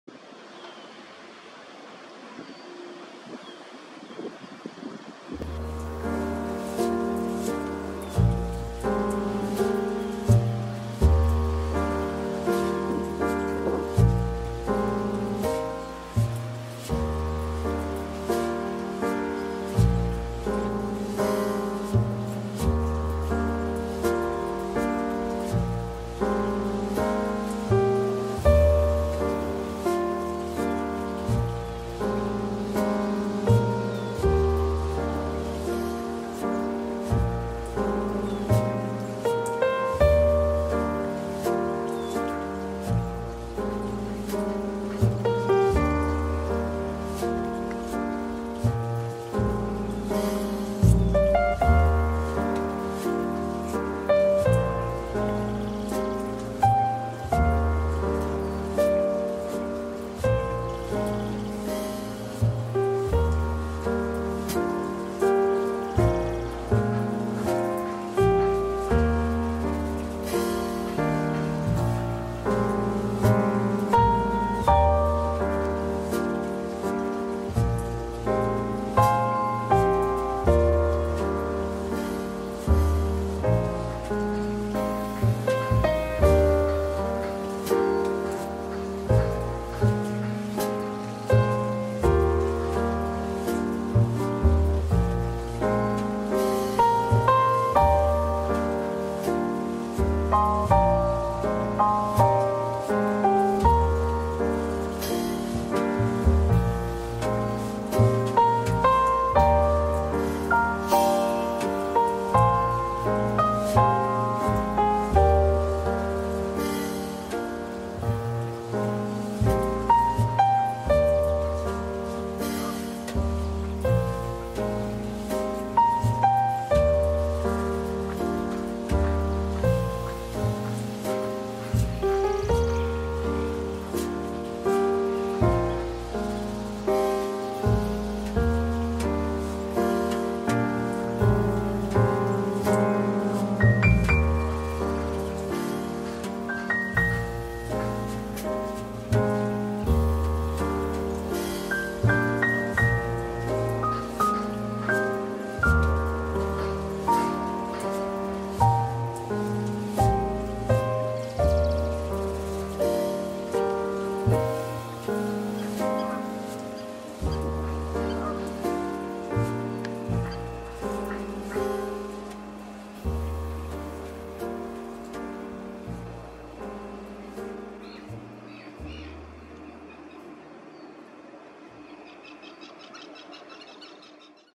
Snowy Egret Fluffing & Dancing - Running around and Flickering for Active Hunting
Snowy Egret running around and flickering in shallow water as if enjoying dancing while similar but larger rival Great Egret moving slowly or just standing still.

Egret & Heron usually stand still and wait patiently or walk slowly to hunt passively. However, Snowy Egret sometimes does active hunting by running around water and flickering to make prey move to catch them afterwards.